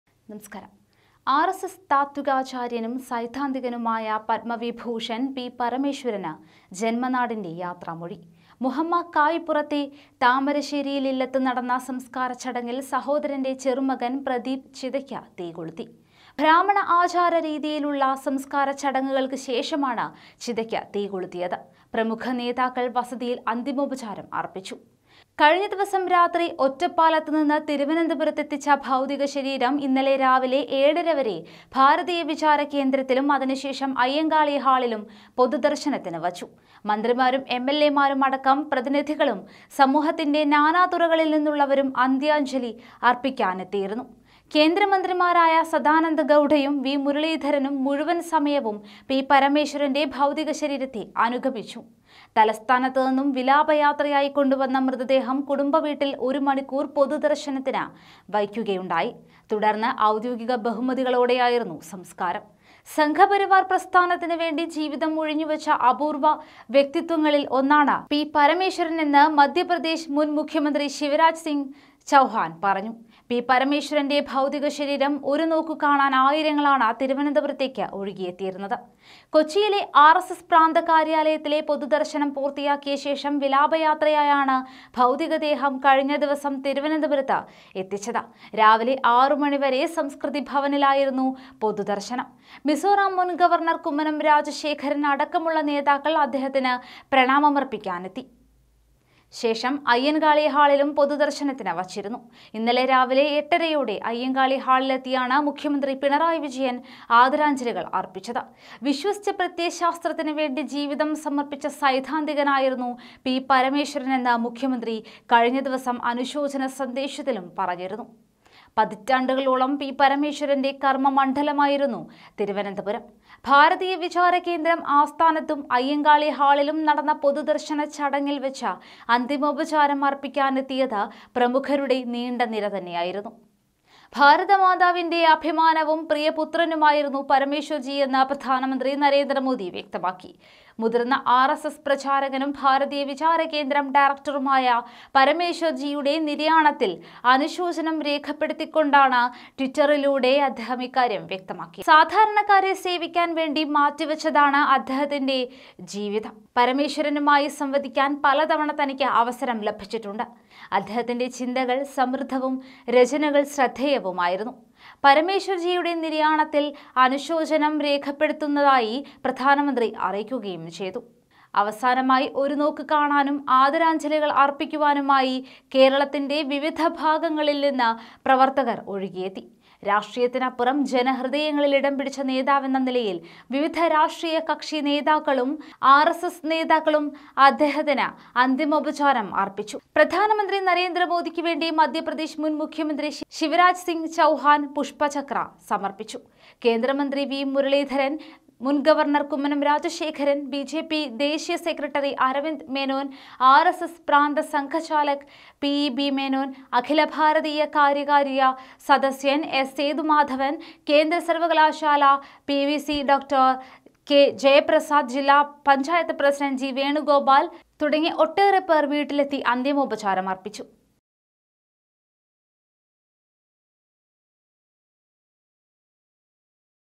പരമേശ്വരന് വികാര നിര്‍ഭരമായ യാത്രാമൊഴി... I p parameswaran
തലസ്ഥാനത്തു നിന്നും വിലാപ യാത്രയായി മൃതദേഹം വൈകിട്ടോടെ താമരശ്ശേരി ഇലത്ത്; ബ്രാഹ്മണ ആചാരപ്രകാരം നടന്ന മരണാനന്തര കർമകൾക്ക് ശേഷം ചിതയ്ക്ക് തീ കൊളുത്തിയത് ചെറുമകൻ പ്രദീപ്; ഭൗതിക ശരീരത്തെ അനുഗമിച്ച് സദാനന്ദ ഗൗഡയും വി മുരളീധരനും, കുമ്മനവും അടങ്ങുന്ന പ്രമുഖർ; ഭാരതാംബയുടെ പുത്രനെന്ന് പ്രധാനമന്ത്രി; വിശ്വസിച്ച പ്രത്യയശാസ്ത്രത്തിന് വേണ്ടി ജീവിതം സമർപ്പിച്ച സൈദ്ധാന്തികനെന്ന് കേരള മുഖ്യനും; ആർ.എസ്.എസ് സൈദ്ധാന്തികൻ പി പരമേശ്വരന് ജന്മനാടിന്റെ യാത്രാമൊഴി!